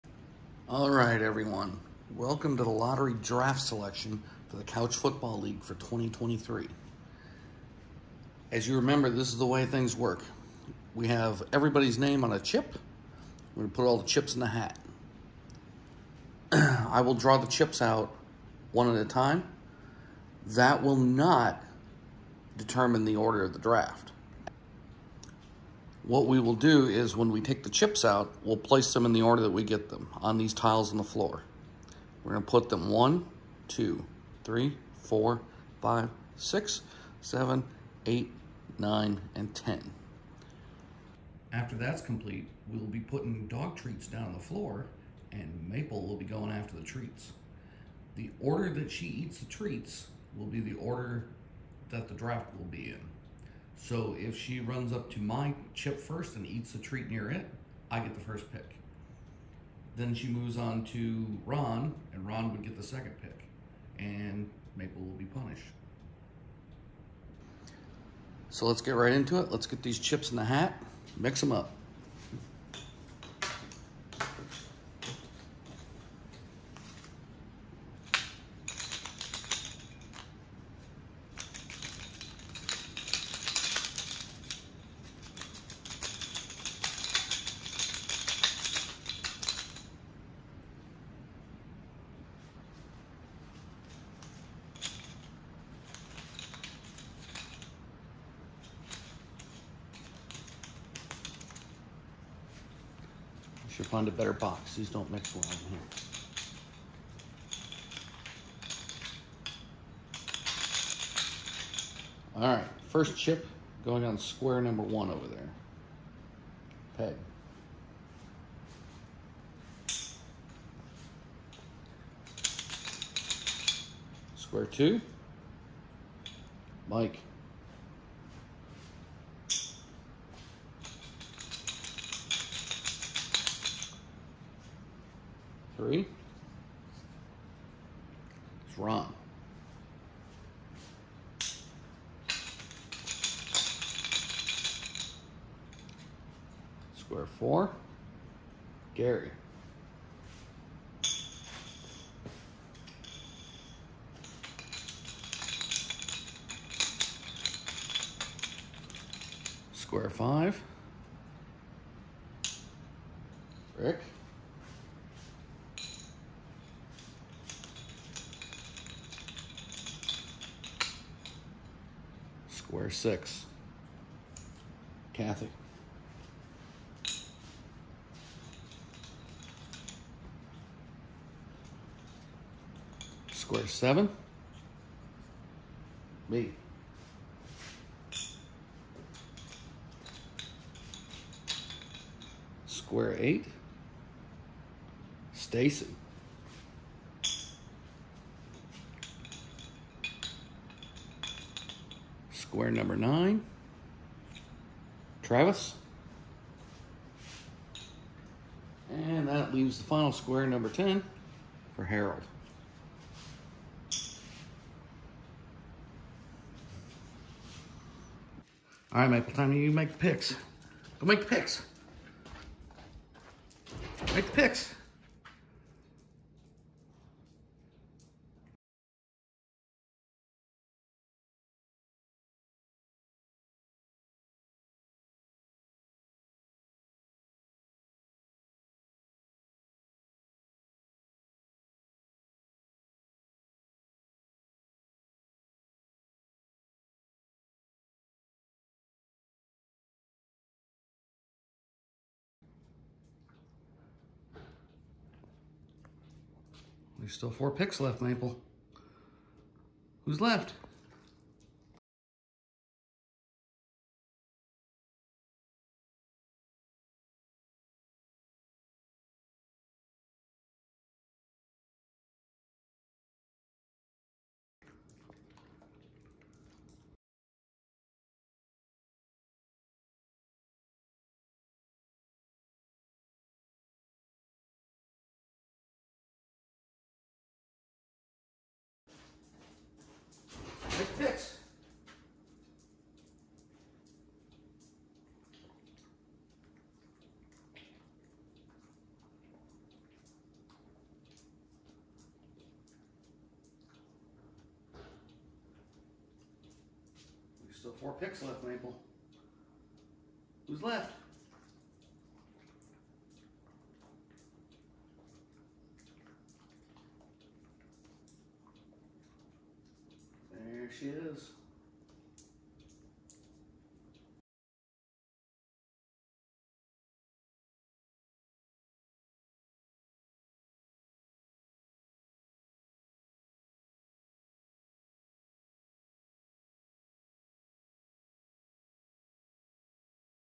Couch Football League Draft Lottery 2023
Maple selects the order that we draft in this season.